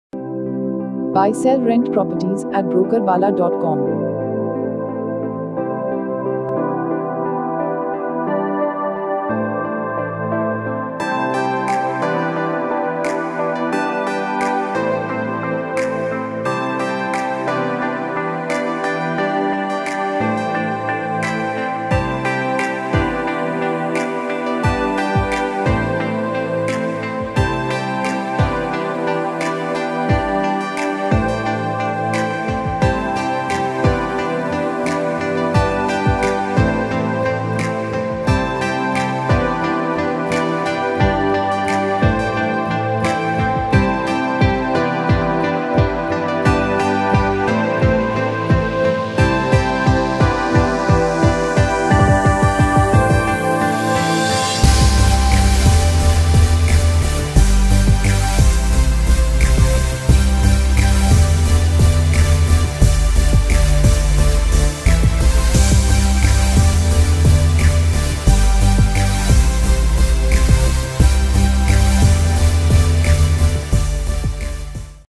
2BHK Full Furnished Flat for Rent in Wakad, Pune
This is a fully furnished 2bhk flat with 2 bathrooms and 2 balconies.

Rent: Rs 22,000 per month